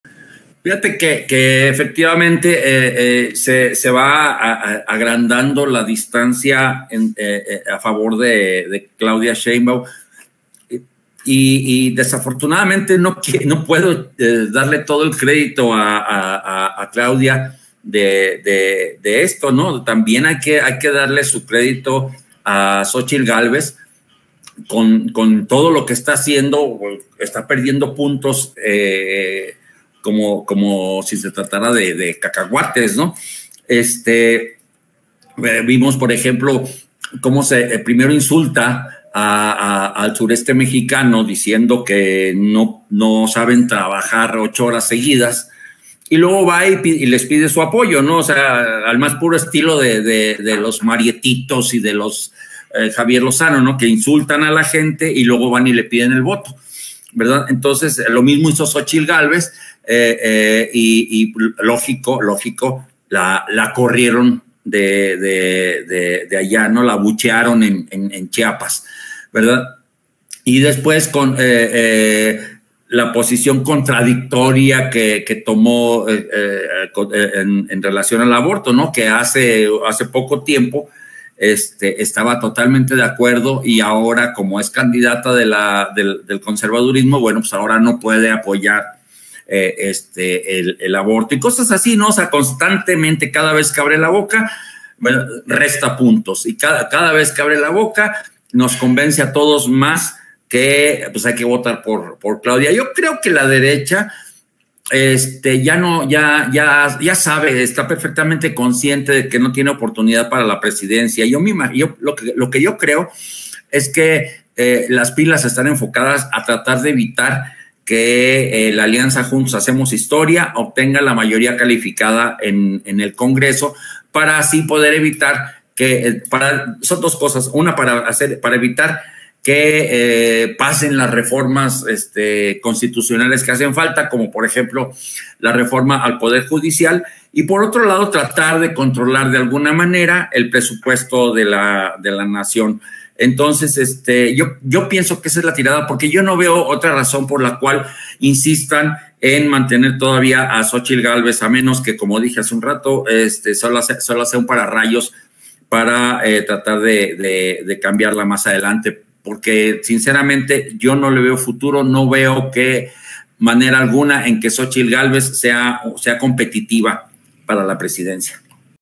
SE VA AGRANDANDO DISTANCIA A FAVOR DE CLAUDIA… CRÉDITO NO ES DE CLAUDIA, SINO DE XÓCHITL: ERICK GTZ
El analista político Erick Gutiérrez considera que eso de que se va agrandando la distancia a favor de Claudia Sheinbaum no es un logro solo de la doctora, sino de la misma Xóchitl Gálvez pues no deja de regarla